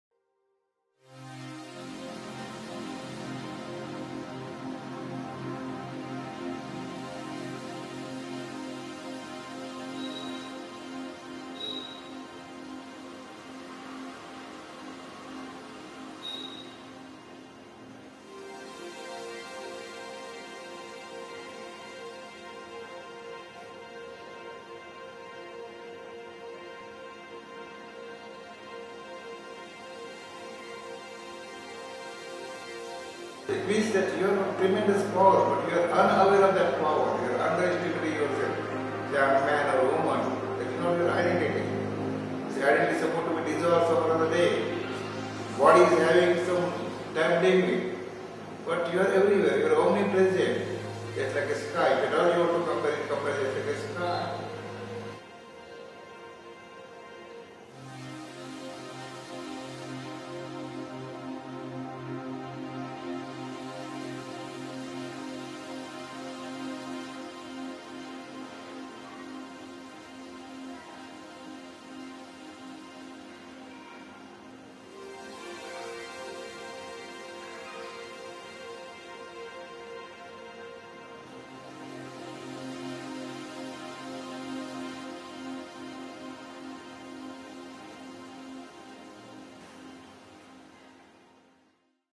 Soi Sans Soi - Ramakant Maharaj
Une introduction à la editon française de 'Selfless Self: Talks with Sri Ramakant Maharaj' publié l'an dernier. Ramakant Maharaj a passé environ 20 ans avec le légendaire Nisargadatta Maharaj. Il enseigne de son ashram à Nashik , Maharashtra. Ce maître réalisé offre le Naam Mantra, initiation à la lignée Inchegiri Navnath de Nisargadatta, Siddharameshwar et autres maîtres importants.